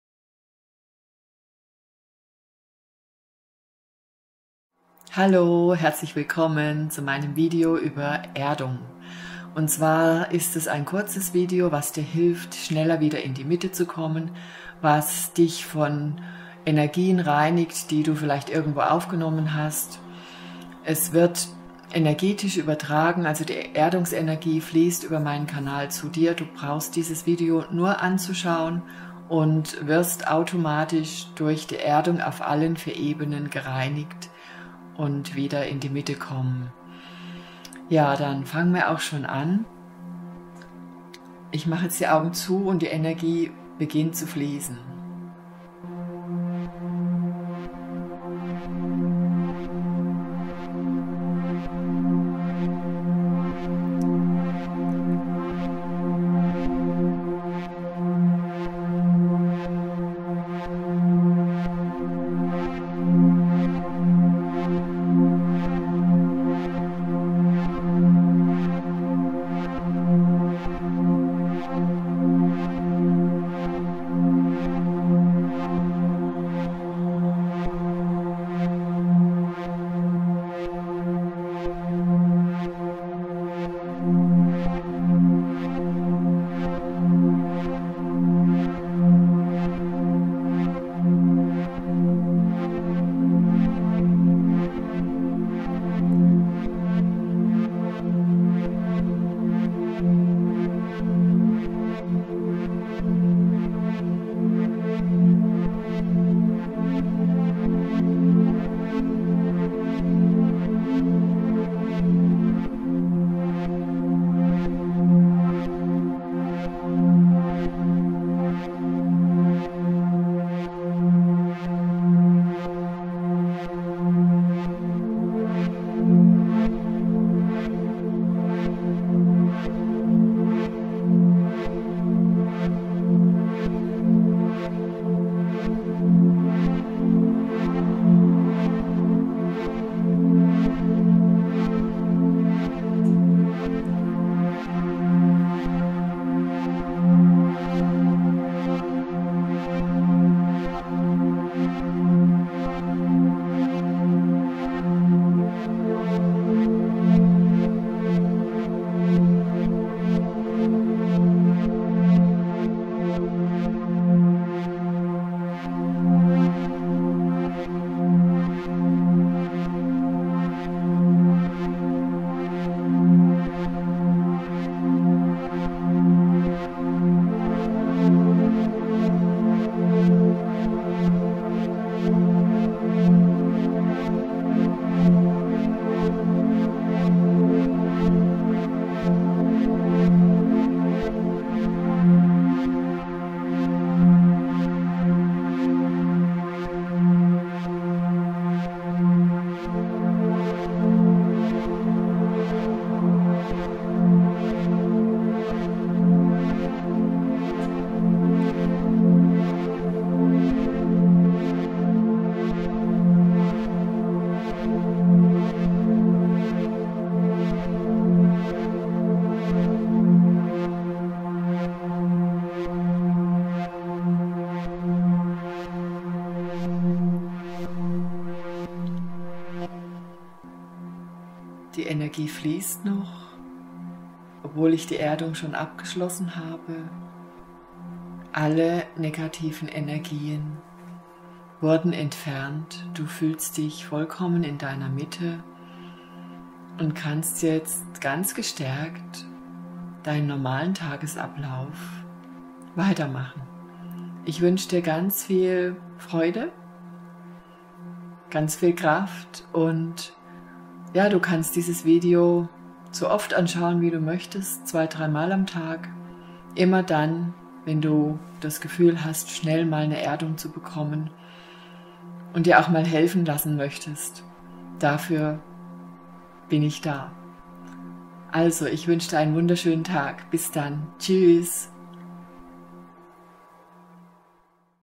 ★ Erdung auf allen vier Ebenen | Smaranaa.eu★
Erdung auf allen vier Ebenen geschieht, wenn du dieses Video anschaust. Die Energien reinigen deine Aura und befreien dich von störenden Einflüssen deines Umfeldes. Wir haben das Video extra kurz gehalten, damit man es schnell mal zwischendurch anschauen kann.